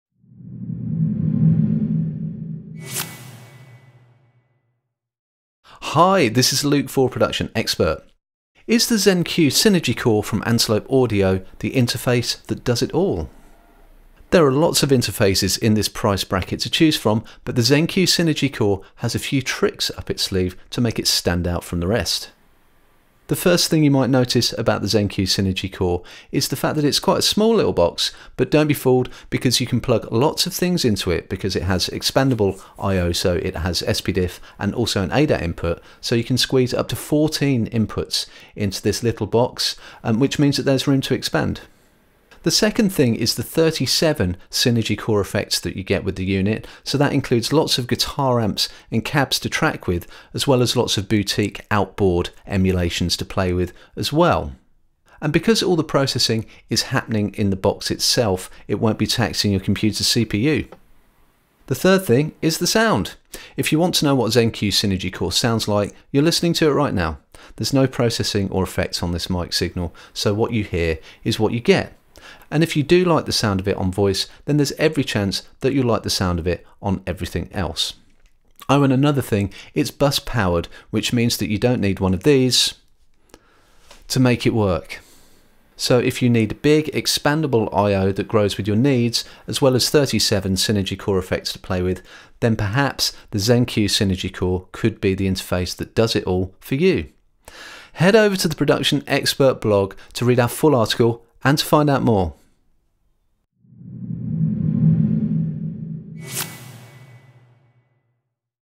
Is The Antelope ZenQ Synergy Core The Interface For A Band?
Buying a piece of studio equipment is always a commitment. How does the small studio owner choose an interface within budget that isn’t going to end up on the shelf?